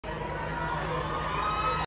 Mark Wahlberg Entering Publix
It was short notice, but hundreds of Mark Wahlberg fans stood in line for hours on Friday 8/25/23 at a Cooper City Publix liquor store for a chance to meet the singer/actor/producer/entrepreneur as he signed bottles of his tequila brand, Flecha Azul, to raise money to support victims of the recent Maui fires.